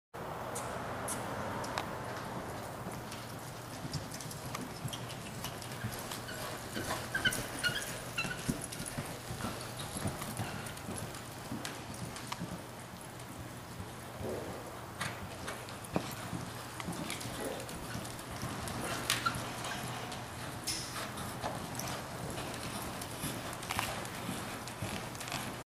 caddy loping barrels
3 yr old filly first time away from home